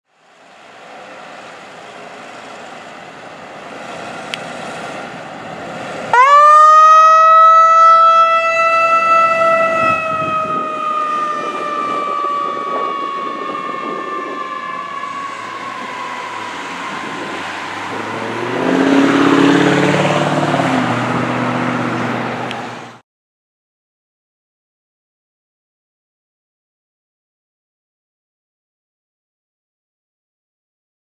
PGFD Ambulance 826 Responding
Prince George’s County Fire & EMS Ambulance 826 responding to a medical call in District Heights, Prince George’s County, MD.